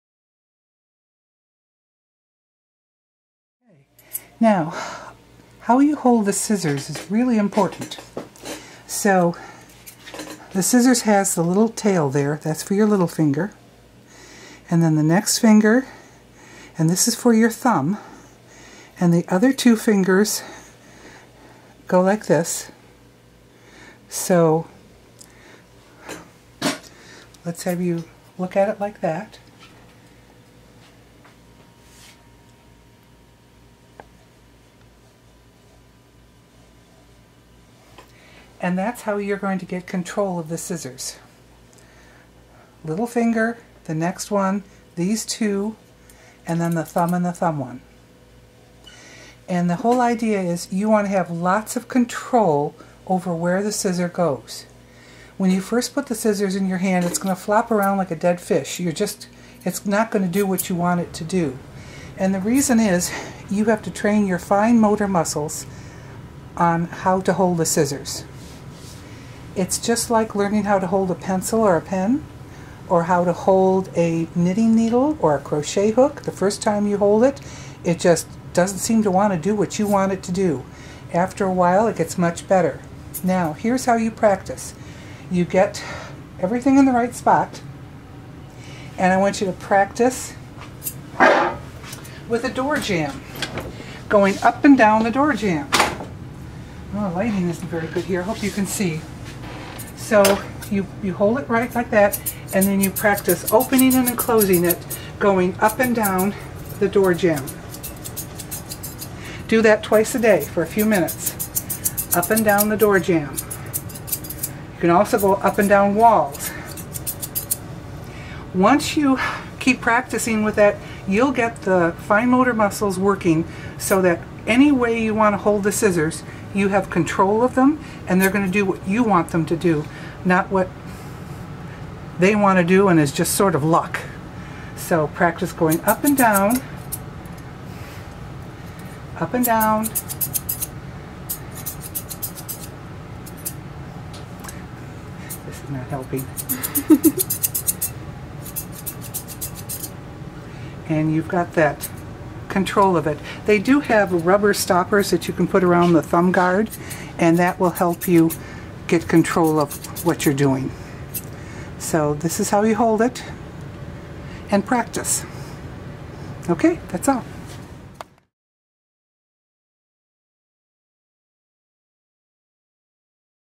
scissor skills Original
Learning how to hold a grooming scissor properly is necessary for  greater scissoring control.  It will take some practice,  but soon you will be using the scissors with confidence and control.